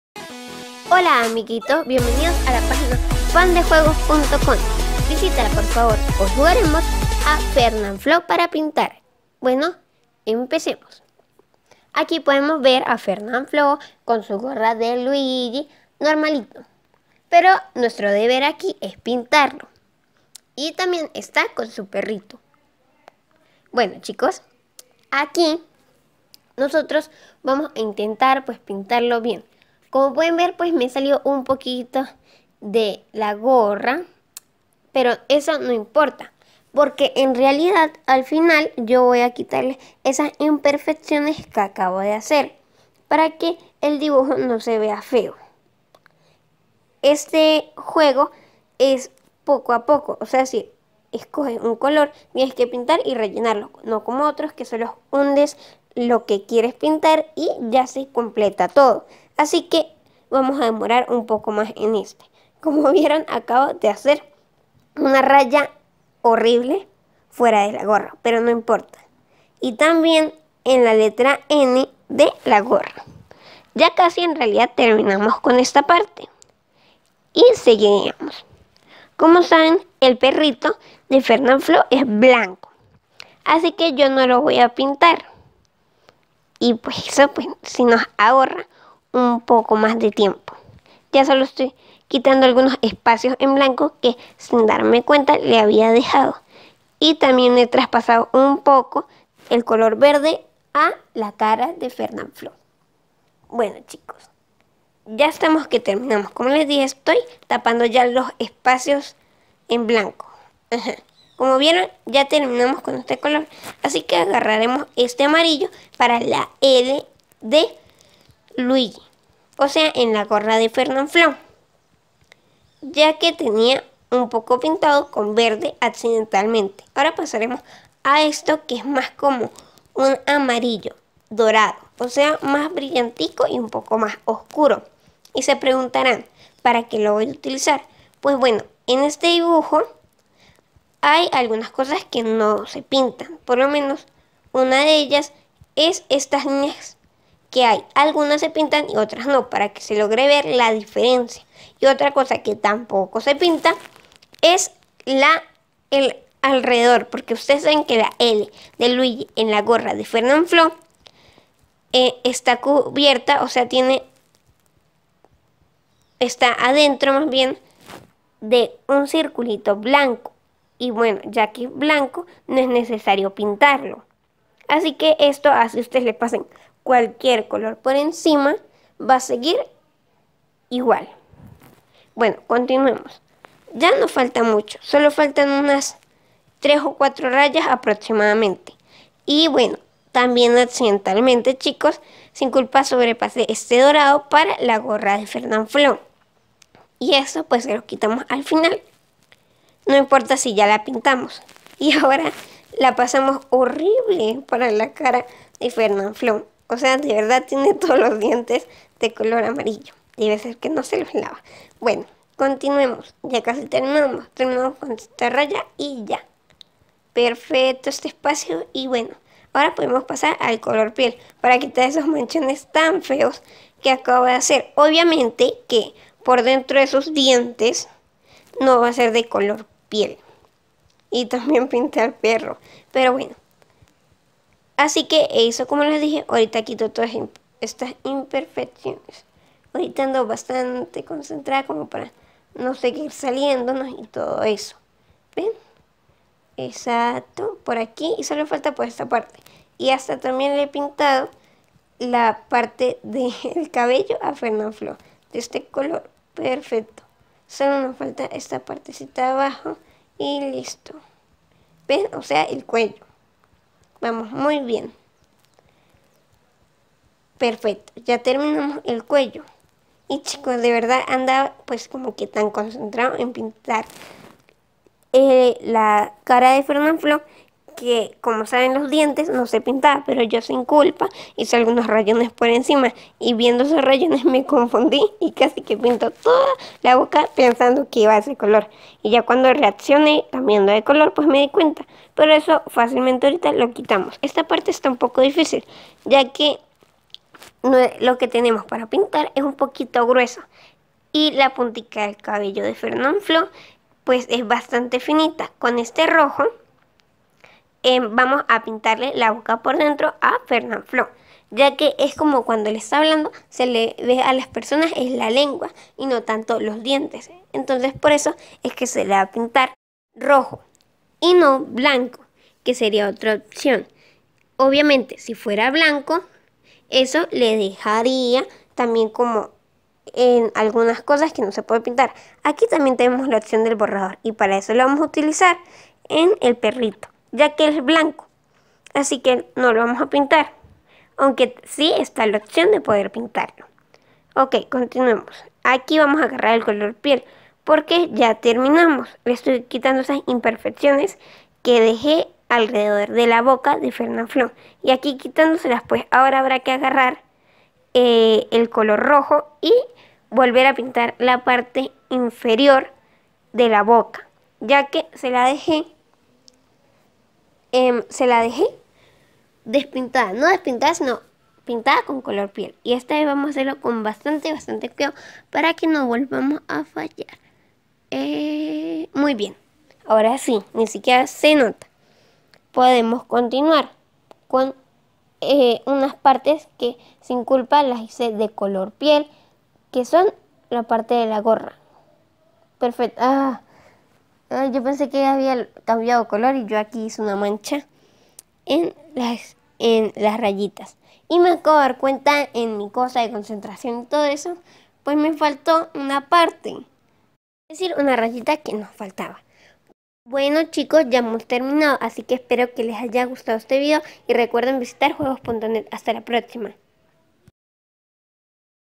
FERNANFLOO PARA PINTAR
os traemos este juego de Fernanfloo que podais encontrar en los enlaces mas abajo por si queréis echarles un vistazo a otros juegos de la misma categoría o jugar a este mismo.
Fernanfloo para pintar: Le vamos a ofrecer un estupendo juego de Fernanfloo para pintar. En este dibujo de Fernanfloo podemos verlo acompañado de su fiel mascota Curly, un lindo perrito que adora nuestro amigo YouTube. Ahora podrás pasar un buen rato pintando a tu YouTube favorito, como es Fernanfloo que con sus locas aventuras jugando a los videojuegos a conquistado a medio mundo. Gracias Fernanfloo por entretenernos.

Bueno y ahora os dejo los links, espero vuestros comentarios y LIKES, os a parecido el juego Fernanfloo para pintar?